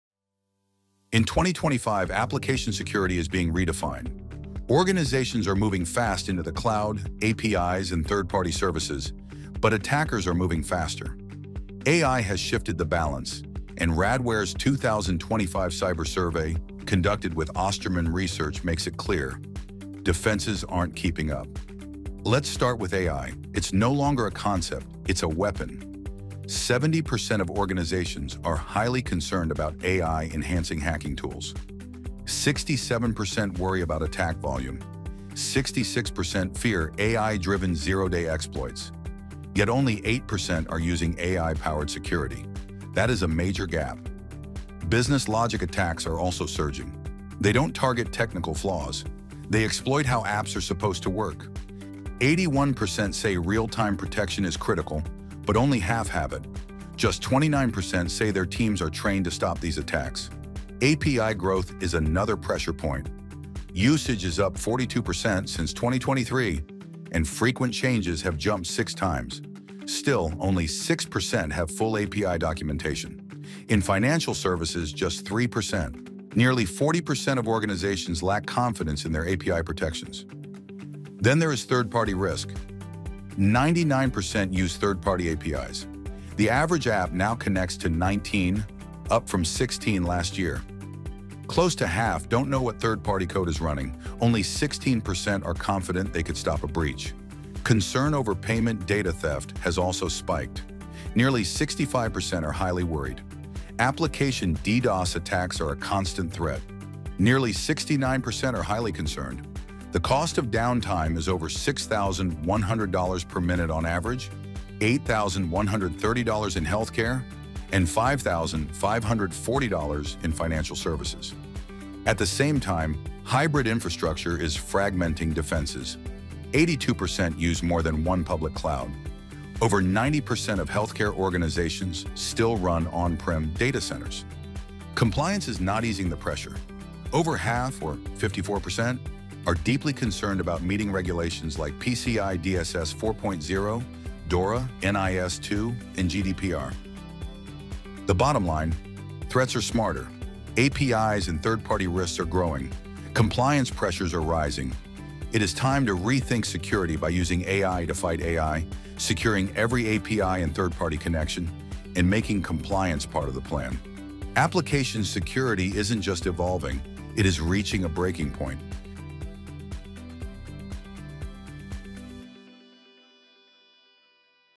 Radware Cyber Survey Uncovers Critical Weaknesses in Application Security Measures
Radware released its new report, 2025 Cyber Survey: Application Security at a Breaking Point. The survey reveals threat areas of rapidly growing concern as organizations’ cyber defenses lag well behind. This includes a major lack of protection against AI threats, as well as API and business logic attacks, among others.